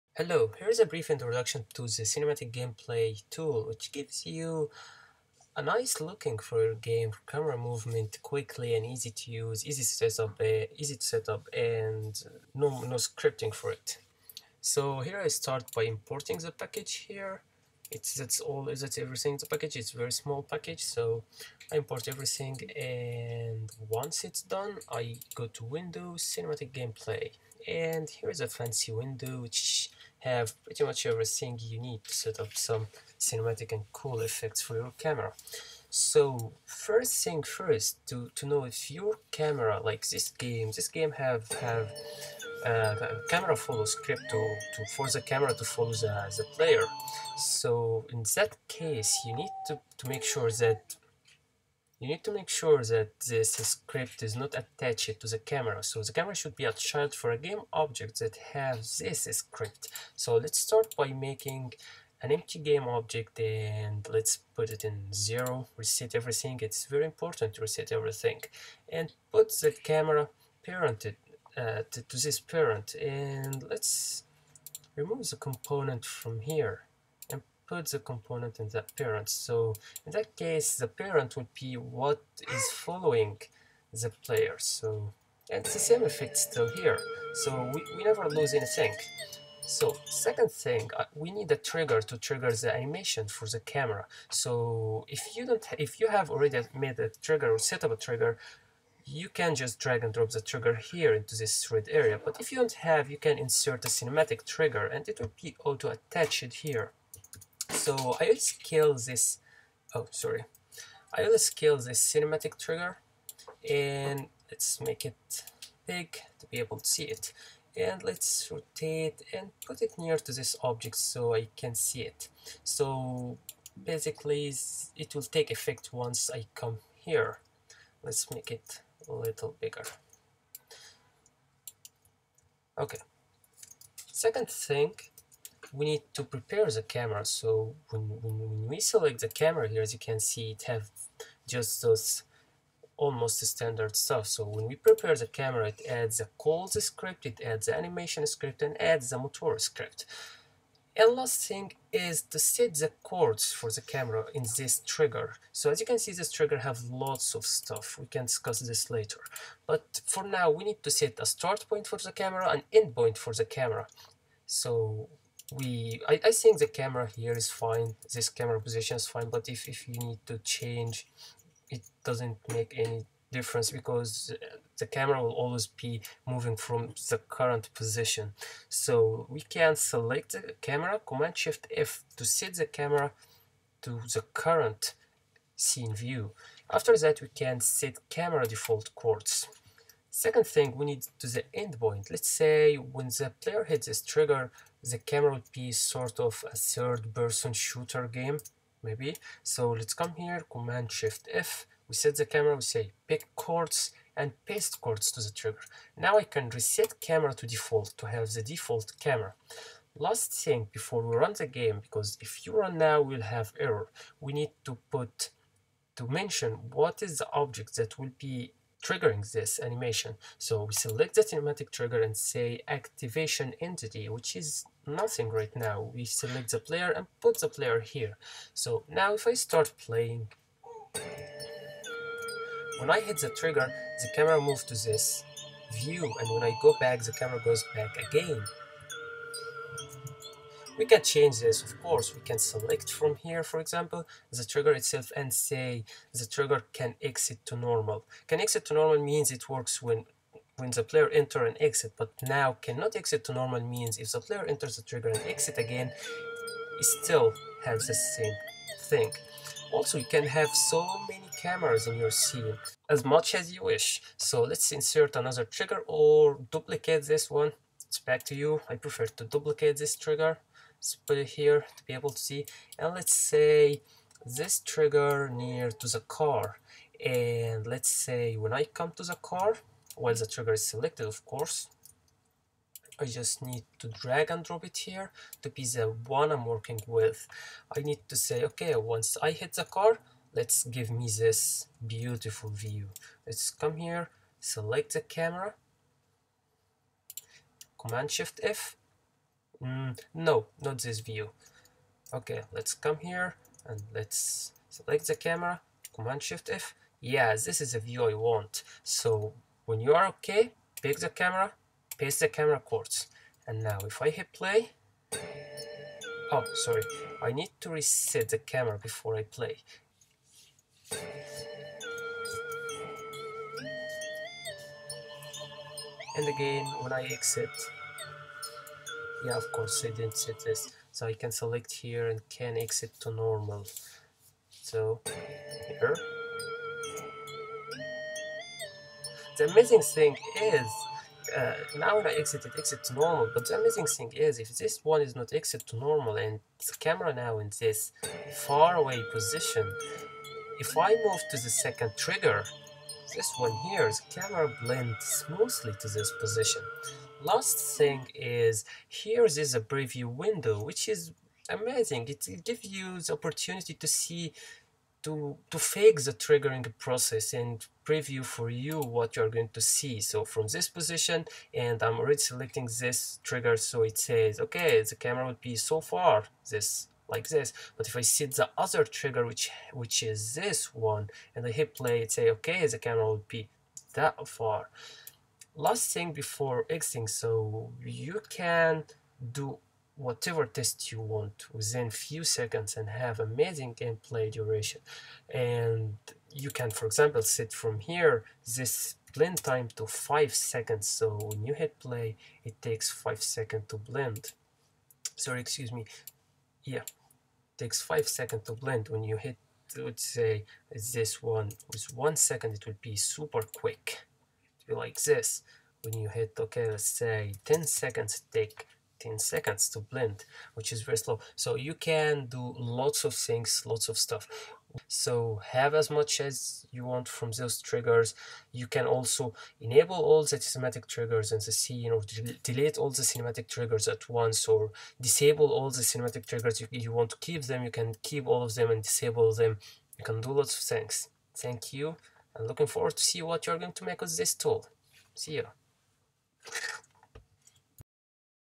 CinematicGameplay Tool tutorial 01 - use with 3d games
Sorry if there were some noise, my kids was playing around ^_^

The Cinematic Gameplay tool allows you to add dynamic camera movements based on triggering areas.
No matter which game genre you are building, as long as there is camera rendering the level, the cinematic camera movements will add a different feeling to the gameplay, starting from full 3d, 2.5d or even 2d games.

With all the embedded functionalities in the cinematic gameplay tool, you will be able to:
- Add dynamic camera movements on runtime.
- use the previewer to watch without entering the game mode.
- Use endless number of cinematic triggers.
- Full C# source, to make it easy to beginners to study it and to professionals to customize it.
- Optimized for the best performance to fit all the supported platforms by Unity.
- Full control on the generated animations, and it's duration.
- Working with Perspective and Orthographic cameras.
- Control over the camera view size per area.